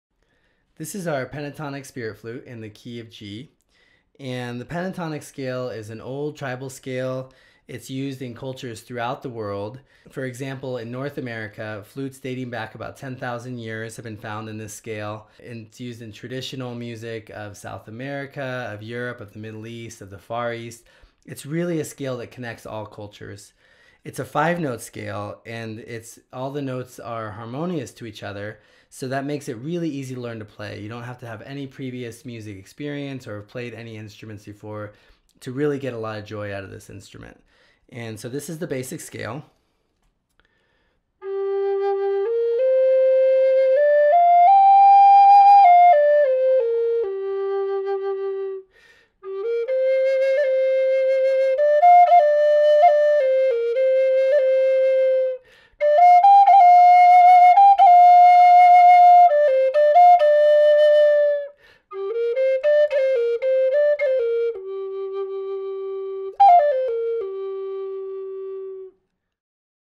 The Spirit Flute - Pentatonic Minor Scale - End Blown - Key of "G"
The pentatonic Spirit Flute in the key of "G" minor has a soothing heart felt voice. Like other pentatonic Spirit Flutes, all the notes in this scale harmonize with each other and no musical experience is needed to create lovely melodies. It has a relaxing and meditative quality but is also responsive and playful. It's a larger flute with a deeper voice than Spirit Flutes in higher keys of "C" or "D". The holes are a bit further apart so we recommend them for those who have played instruments before or for beginners with larger hands.

This flute fills a space with life, creating a feeling of peace and joy. It's accurately tuned so it can be played along with other instruments and is a wonderful accompaniment for all types of hand drums and stringed instruments like the guitar, ukulele, and cello. It also sounds lovely with piano.

Spirit Flutes are crafted from renewable and domestically grown walnut and sealed with non-toxic oils, giving them a warm, resonant, full voice. All Spirit Flutes are crafted in the United States and we are proud to be a part of a long tradition of American craftsmanship.

This flute is 19" long.